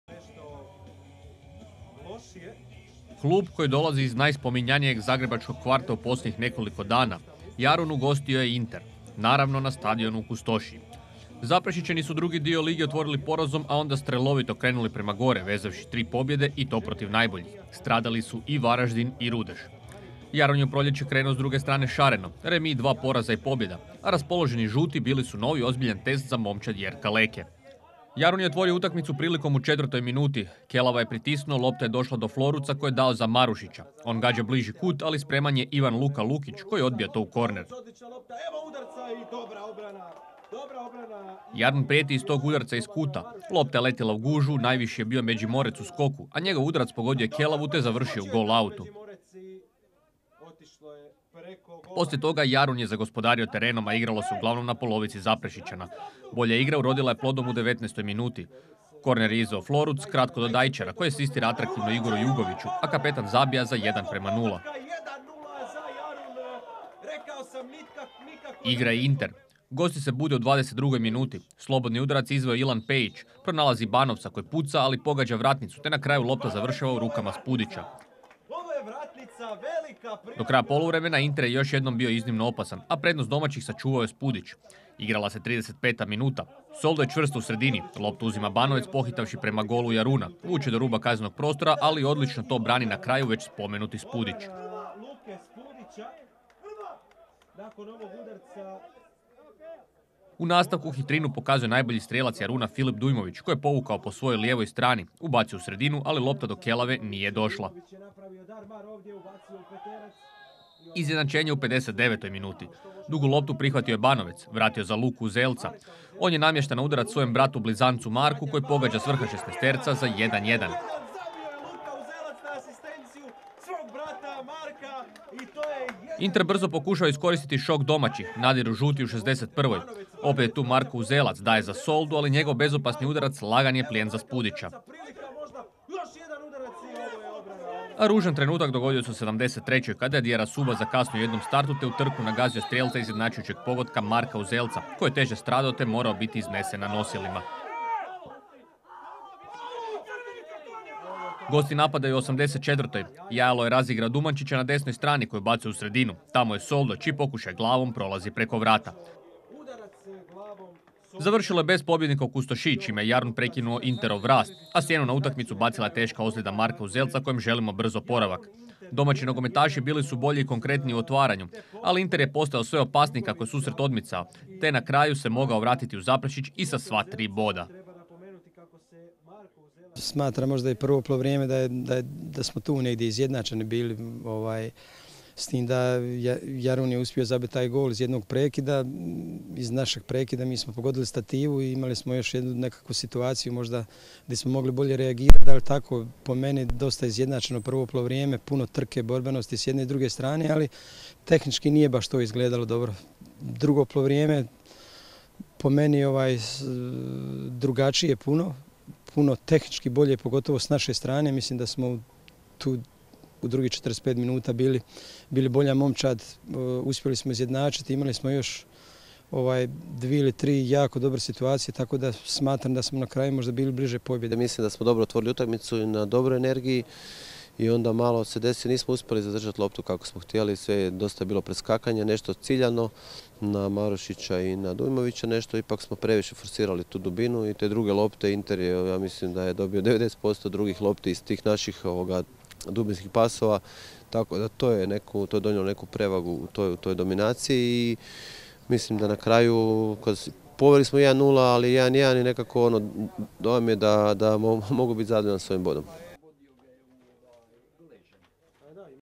JARUN vs INTER-ZAPREŠIĆ 1:1 (20. kolo, Druga HNL 21/22)
Sažetak utakmice 20. kola Druge lige između Jaruna i Inter-Zaprešića (1:1).